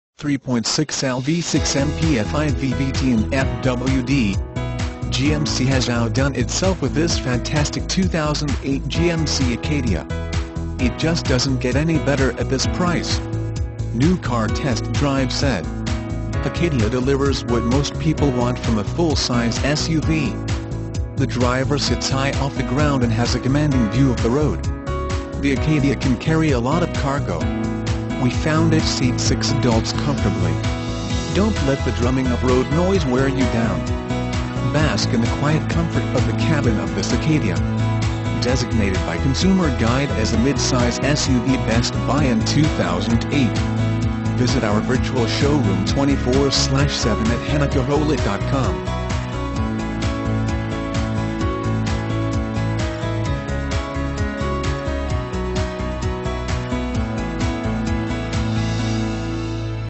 2008 GMC ACADIA Austin, TX 8J185756
2008 GMC ACADIA Austin, TX
Stock# 8J185756

Henna Chevrolet
8805 North IH-35

3.6L V6 MPFI VVT and FWD. GMC has outdone itself with this fantastic 2008 GMC Acadia. It just doesn't get any better at this price! New Car Test Drive said,  ...Acadia delivers what most people want from a full-size SUV. The driver sits high off the ground and has a commanding view of the road. The Acadia can carry a lot of cargo. We found it seats six adults comfortably...  Don't let the drumming of road noise wear you down. Bask in the quiet comfort of the cabin of this Acadia. Designated by Consumer Guide as a Midsize SUV Best Buy in 2008. Visit our virtual showroom 24/7 @ hennachevrolet.com.